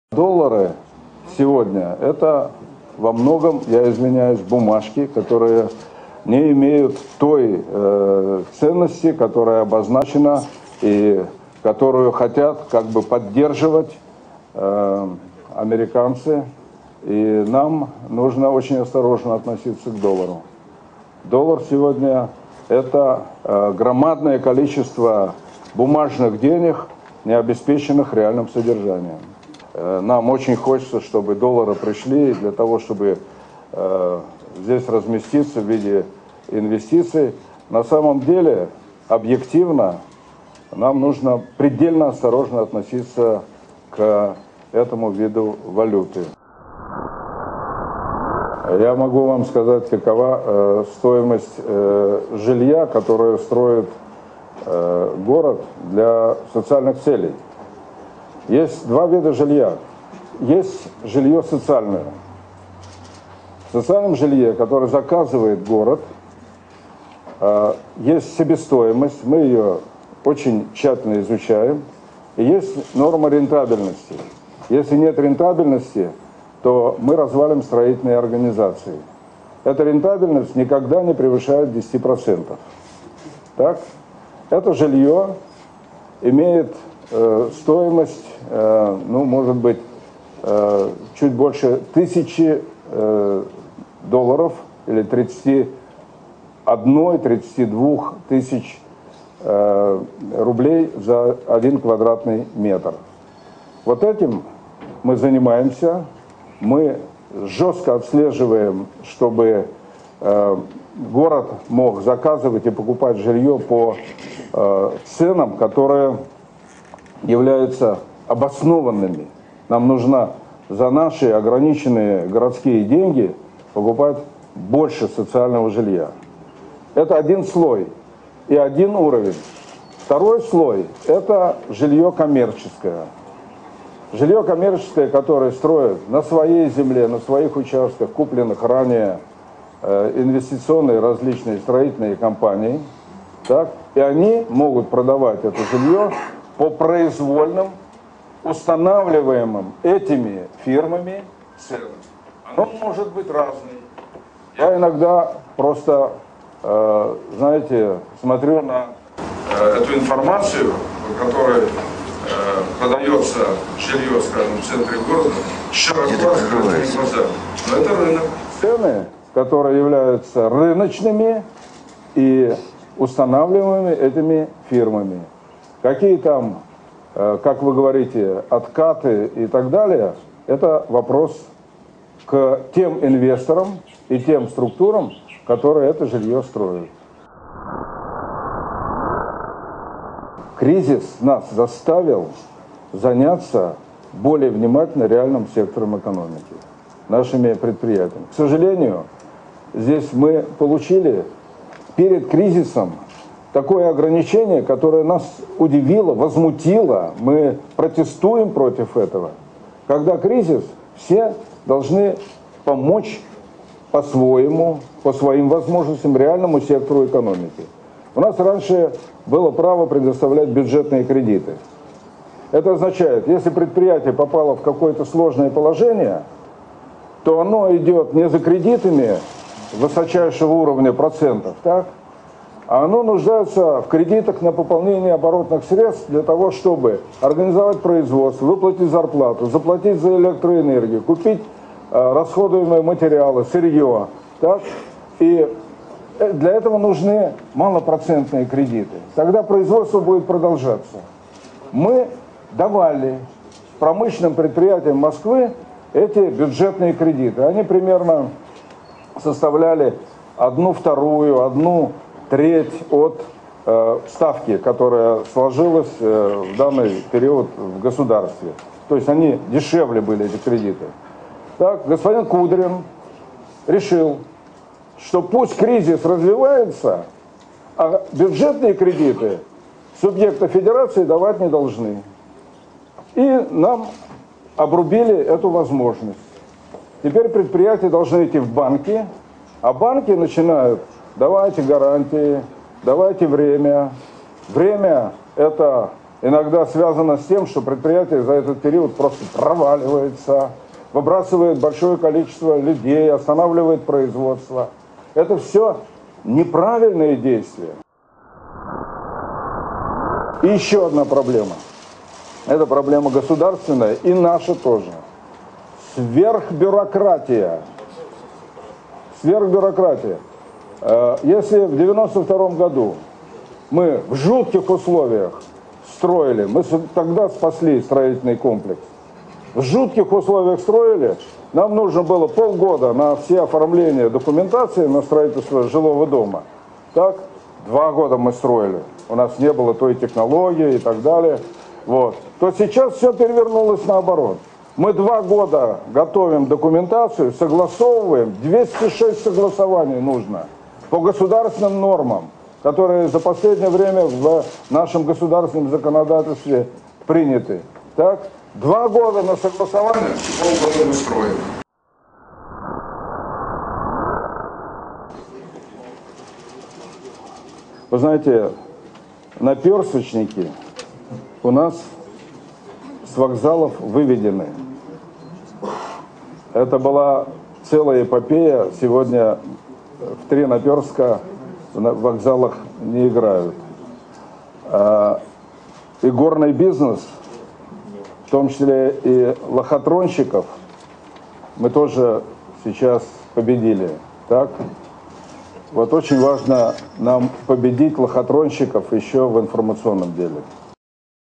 Ю. Лужков: "Доллары - это, я извиняюсь, бумажки"
Мэр Москвы о ценах на жилье, решении Кудрина, лохотронщиках и бюрократах